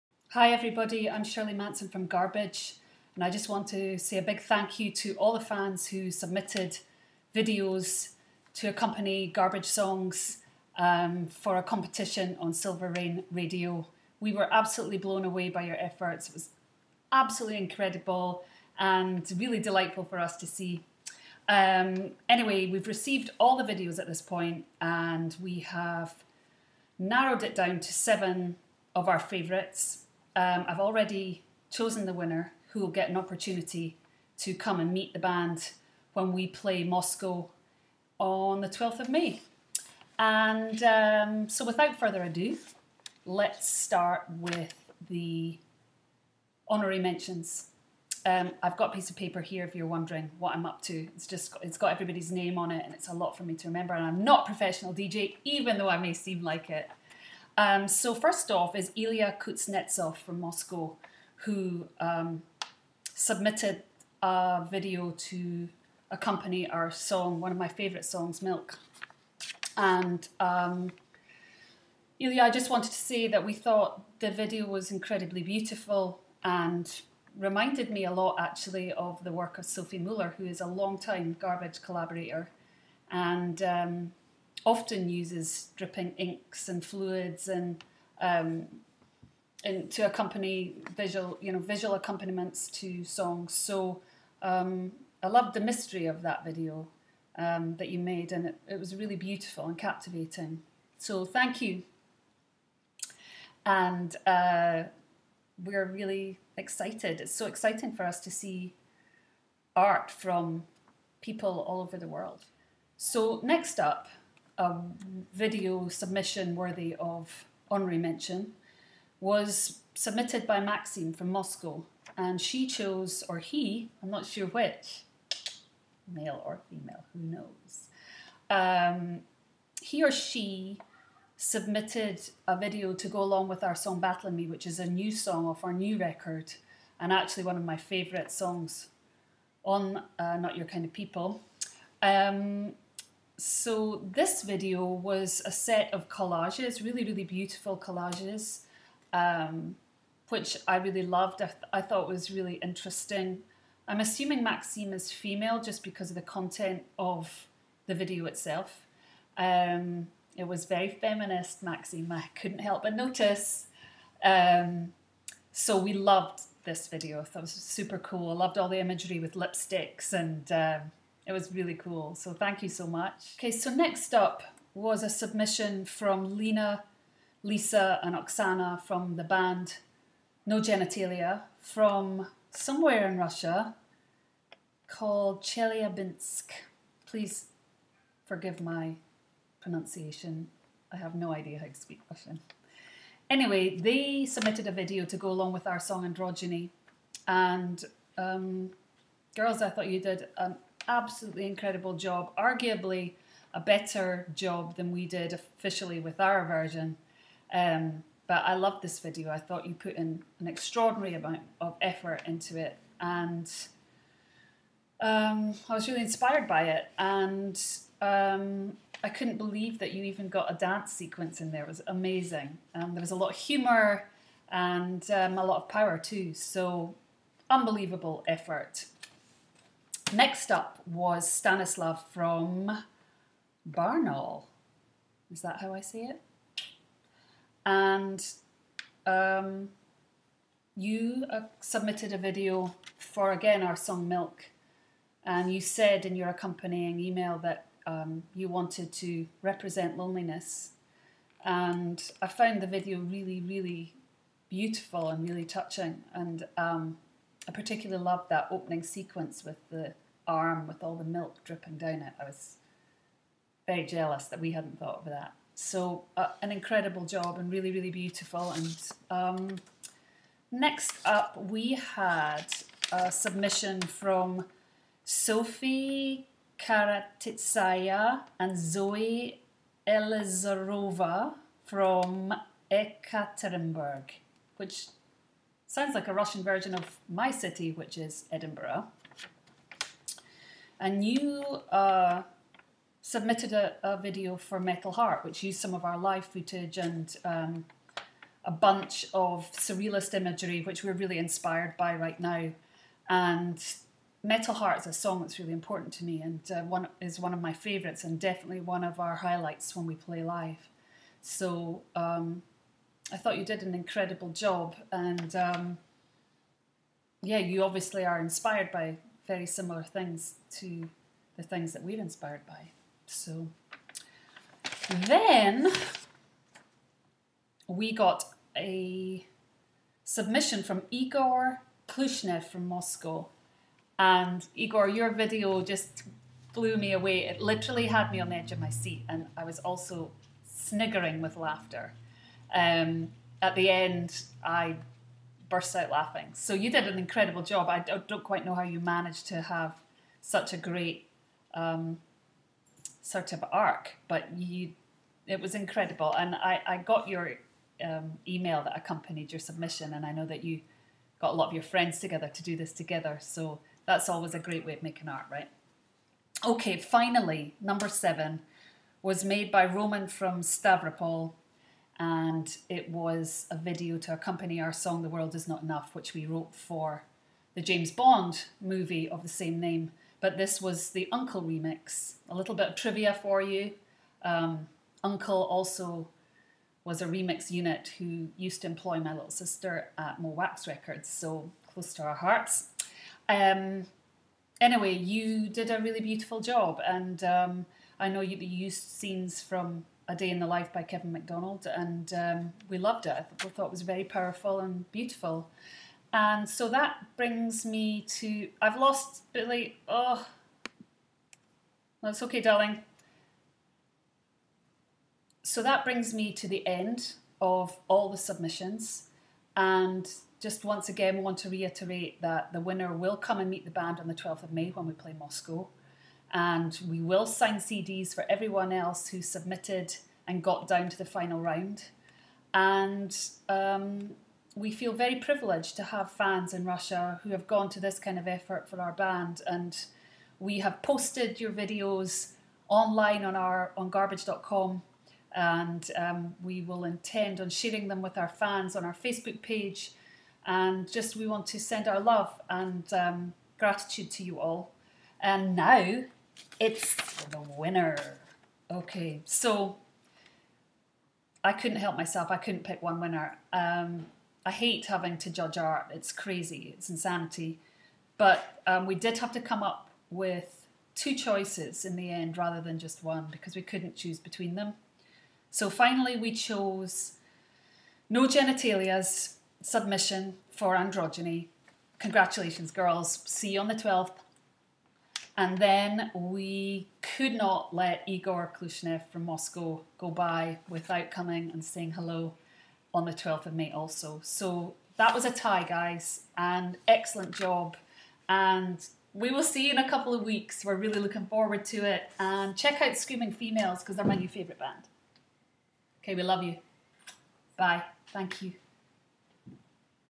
Shirley Manson for Silver Rain Radio
Видеообращение Ширли Мэнсон к участником конкурса на лучший любительский клип.
Shirley Manson made this video message for the participants of russian video contest. May 2012.